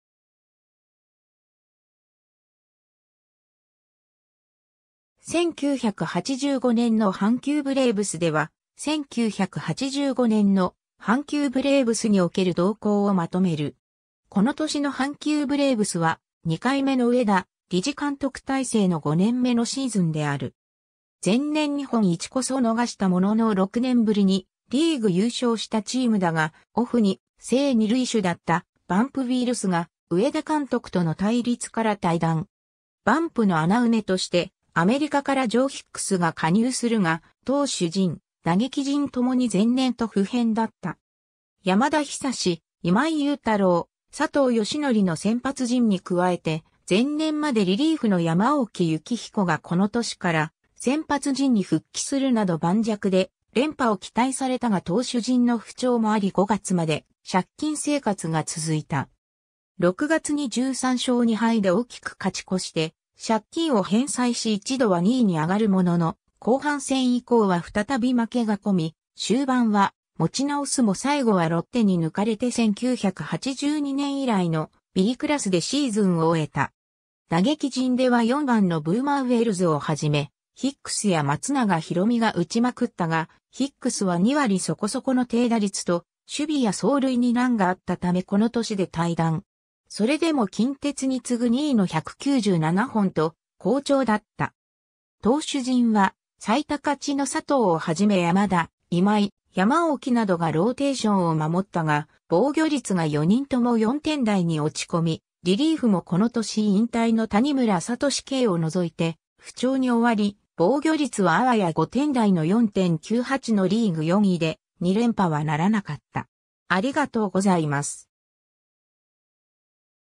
1985年の阪急ブレーブス
各年のオリックス・バファローズ
1985年の阪急ブレーブス（1985ねんのはんきゅうブレーブス）では、1985年の阪急ブレーブスにおける動向をまとめる。
 この年の阪急ブレーブスは、2回目の上田利治監督体制の5年目（通算10年目）のシーズンである。
 前年日本一こそ逃したものの6年ぶりにリーグ優勝したチームだが、オフに正二塁手だったバンプ・ウィルスが上田監督との対立から退団。
バンプの穴埋めとしてアメリカからジョー・ヒックスが加入するが、投手陣・打撃陣ともに前年と不変だった。
山田久志・今井雄太郎・佐藤義則の先発陣に加えて、前年までリリーフの山沖之彦がこの年から先発陣に復帰するなど盤石で、連覇を期待されたが投手陣の不調もあり5月まで借金生活が続いた。
6月に13勝2敗で大きく勝ち越して借金を返済し一度は2位に上がるものの後半戦以降は再び負けが込み、終盤は持ち直すも最後はロッテに抜かれて1982年以来のBクラスでシーズンを終えた。
打撃陣では4番のブーマー・ウェルズをはじめヒックスや松永浩美が打ちまくったが、ヒックスは2割そこそこの低打率と守備や走塁に難があったためこの年で退団。
それでも近鉄に次ぐ2位の197本と好調だった。
投手陣は最多勝の佐藤をはじめ山田・今井・山沖などがローテーションを守ったが防御率が4人とも4点台に落ち込み（特に今井は4.99だった）、リリーフもこの年引退の谷村智啓を除いて不調に終わり防御率はあわや5点台の4.98のリーグ4位で、2連覇はならなかった。